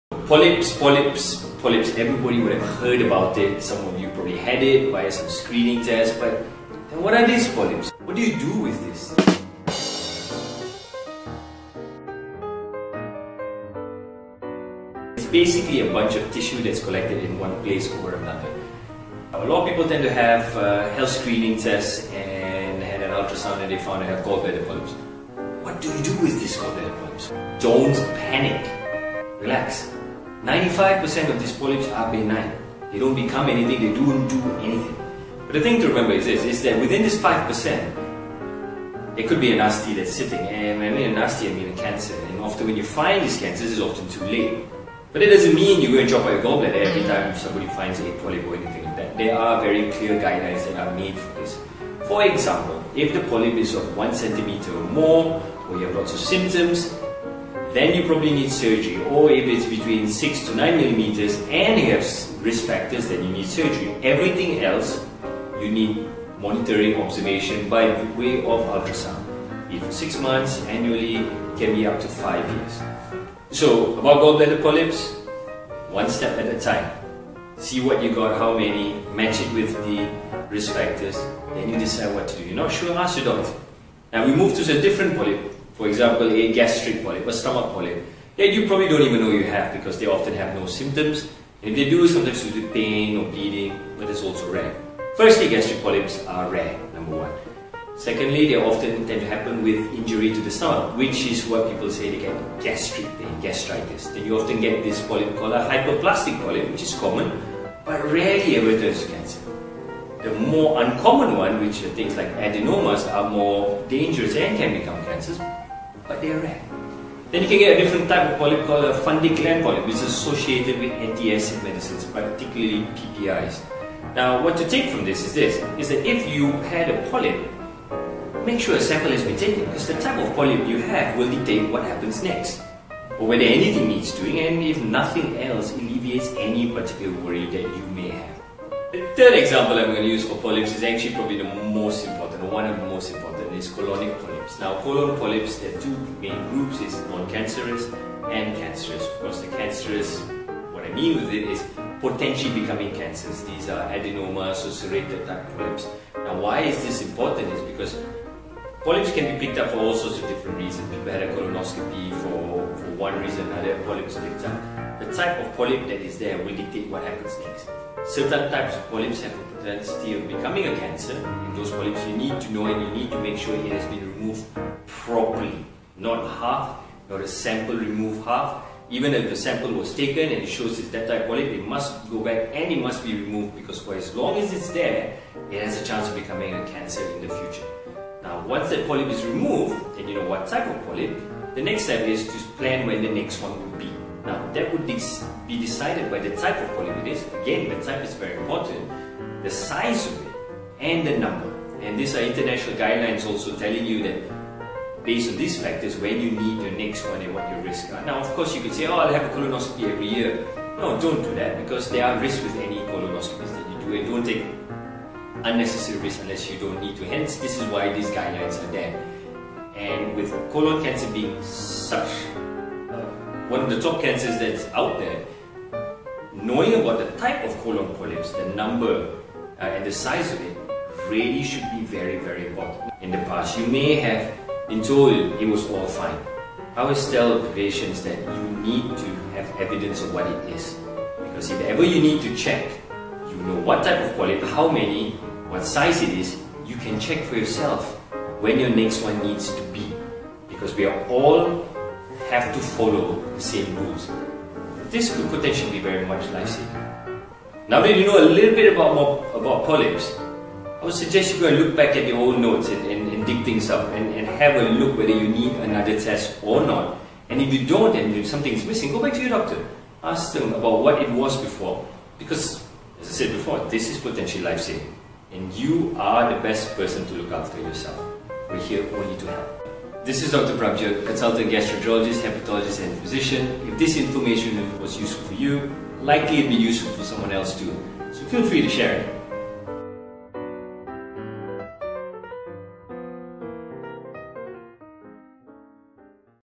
What are Polyps? - by Doctor Prabhjot
Doctor Prabhjot shares about Polyps and why you need to be aware.